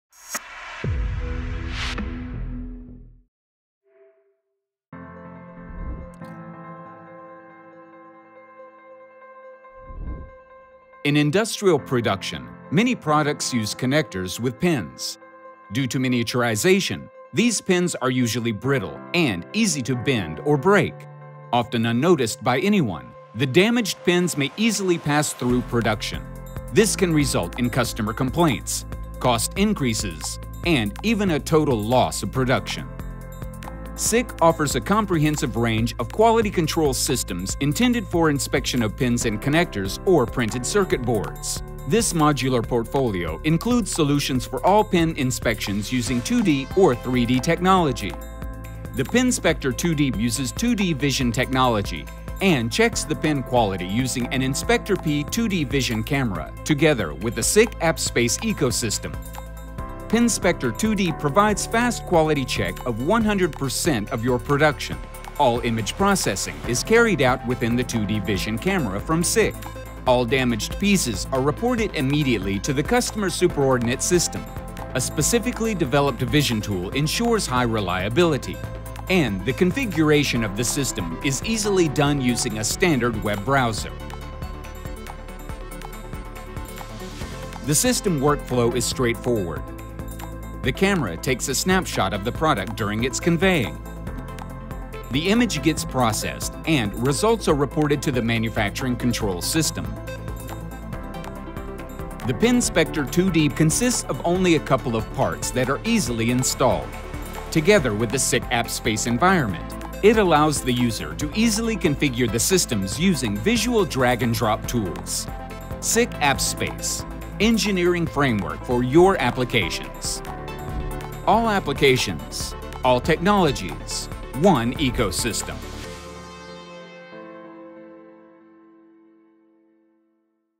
Pinspector 2D: Verifying the presence of pins and their position | SICK AG
Pinspector 2D is an easy-to-set compact quality control system designed for various connector inspections, with primary focus on pin presence and position measurement. Thanks to its advanced inspections, the system also detects and sorts blobs according to pre-set parameters, reads and verifies 1D and 2D codes, provides OCR, OCV functions and measures dimensions. The specialization in the 2D pin inspection with the combination of additional inspections makes it a great match for demanding applications.